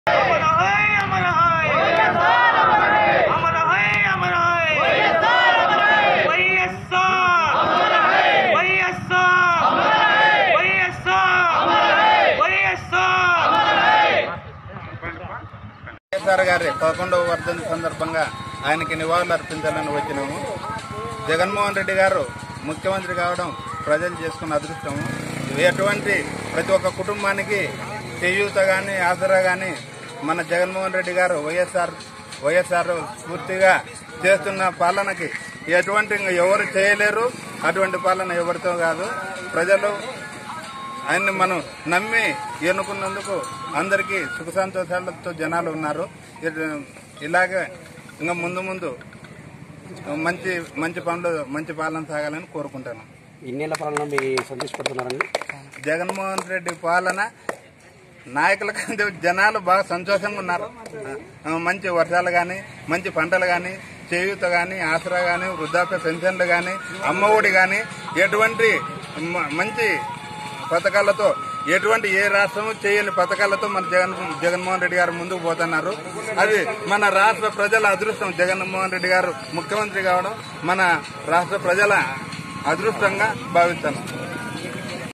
అనంతపురం జిల్లా పెద్దవడుగురులో వై.ఎస్.ఆర్ 11వ వర్ధంతి వేడుకలు ||VS9NEWS
Watch:
అనంతపురం జిల్లా పెద్దవడుగూరులో వై.ఎస్.ఆర్. వర్ధంతి వేడుకలు ఘనంగా నిర్వహించారు.